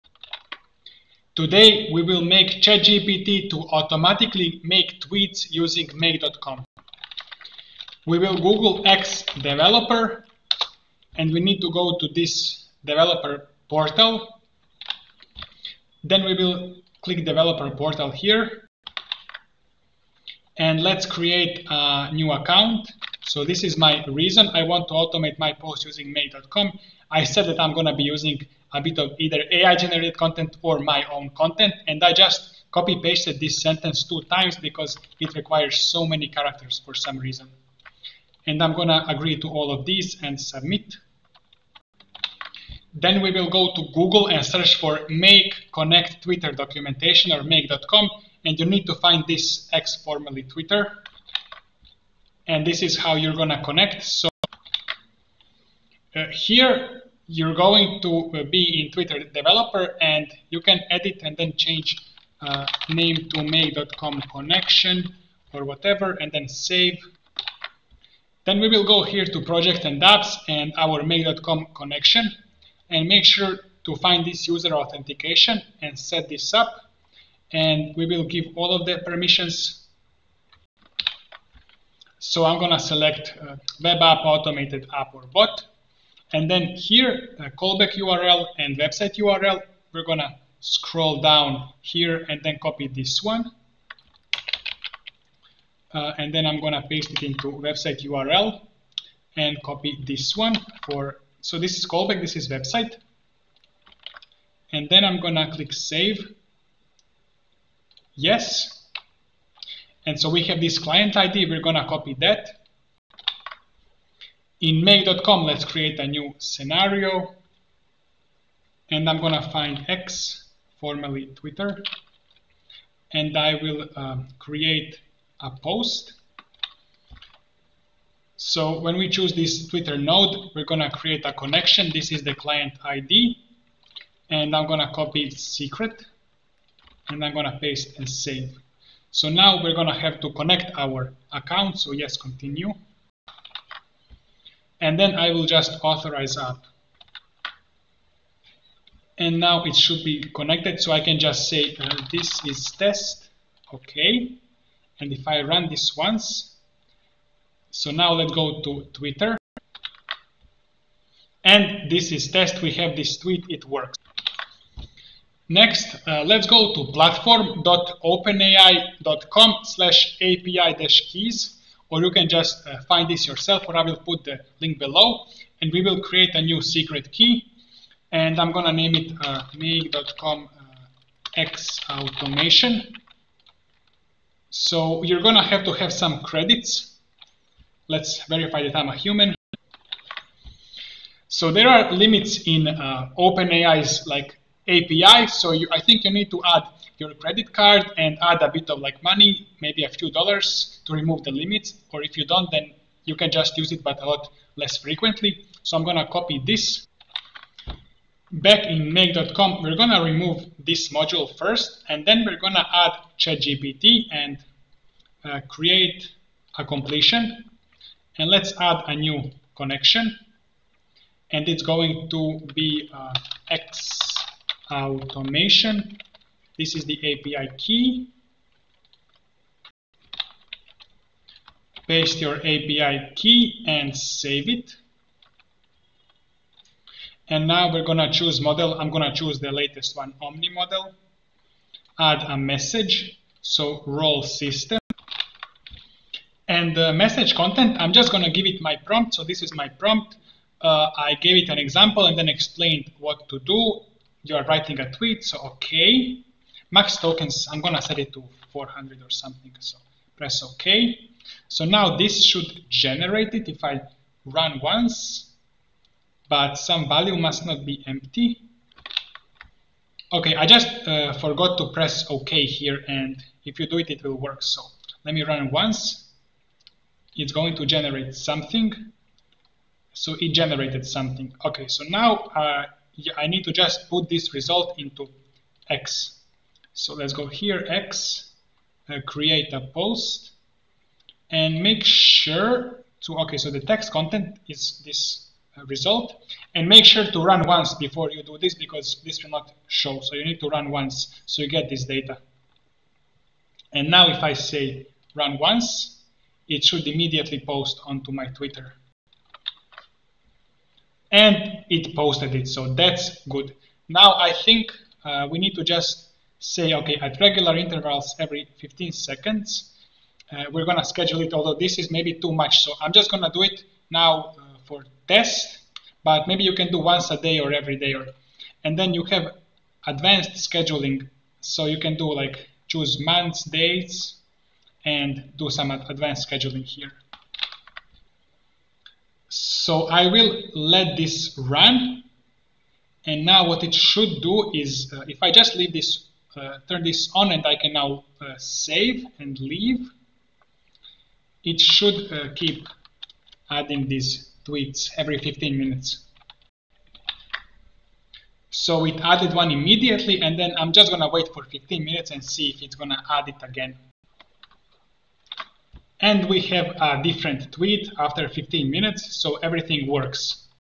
Twitter (X) + ChatGPT + Make.com: Automation
Join my NEW Skool - Learn AI Automation & Chatbots Automate tweets to automatically generate and post with chatgpt with this tutorial.